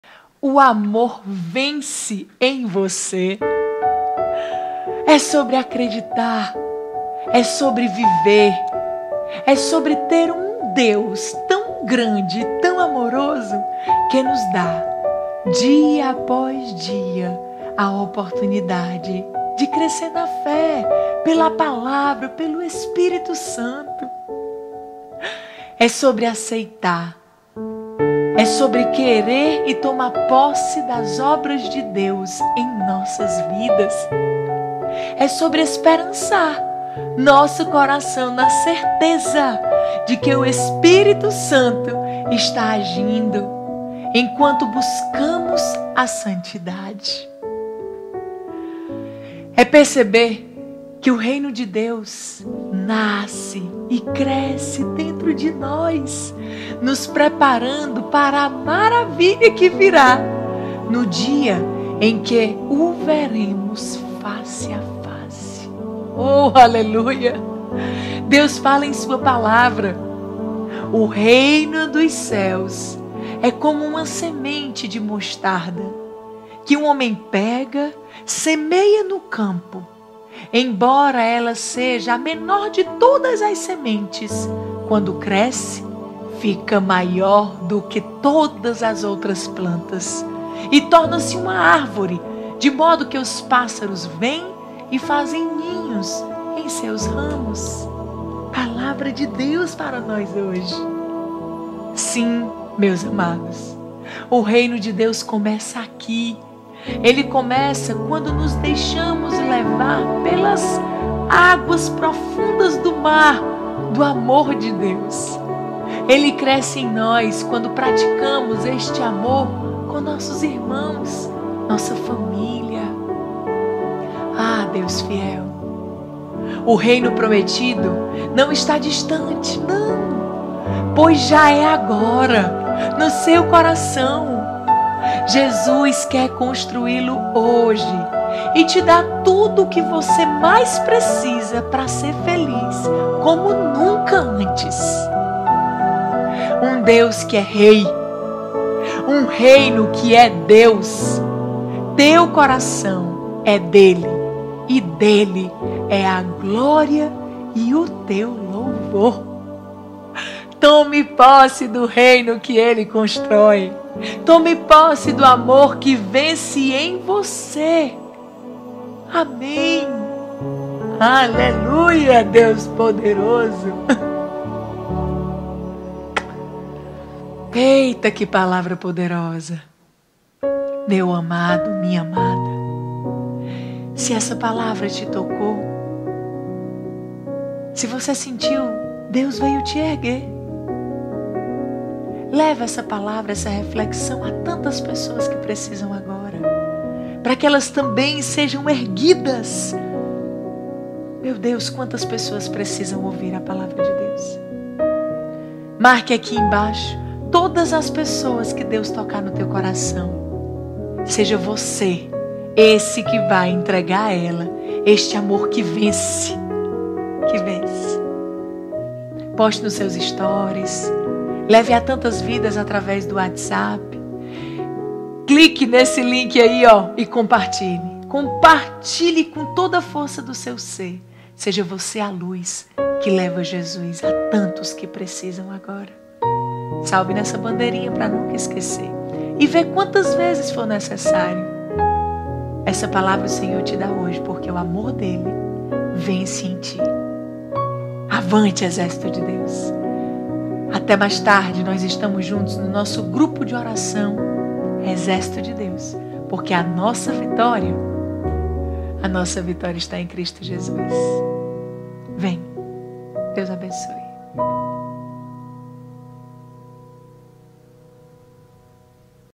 Palavra do dia / O AMOR VENCE EM VOCÊ
Comente o que Deus falou em seu coração. Marque as pessoas que Ele te tocar. Compartilhe a Palavra de hoje em todas as redes!

Naquele tempo, 31Jesus contou-lhes outra parábola: “O Reino dos Céus é como uma semente de mostarda que um homem pega e semeia no seu campo. 32Embora ela seja a menor de todas as sementes, quando cresce, fica maior do que as outras plantas. E torna-se uma árvore, de modo que os pássaros vêm e fazem ninhos em seus ramos”. 33Jesus contou-lhes ainda uma outra parábola: “O Reino dos Céus é como o fermento que uma mulher pega e mistura com três porções de farinha, até que tudo fique fermentado”. 34Tudo isso Jesus falava em parábolas às multidões. Nada lhes falava sem usar parábolas, 35para se cumprir o que foi dito pelo profeta: ‘Abrirei a boca para falar em parábolas; vou proclamar coisas escondidas desde a criação do mundo’.